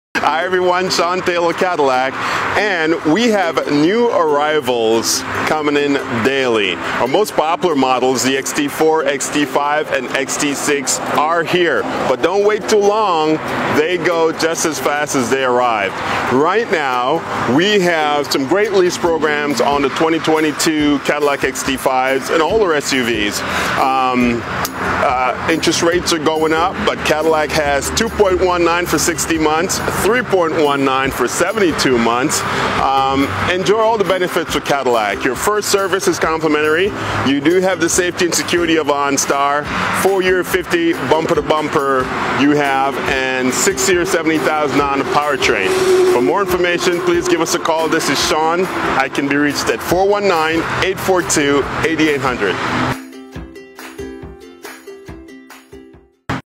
Cadillac Specials
We know the interest rates are going up BUT Cadillac is currently offering amazing lease specials on our SUV lineup and 2.19%* | 60 months and 3.19%* 72 months financing! 🥂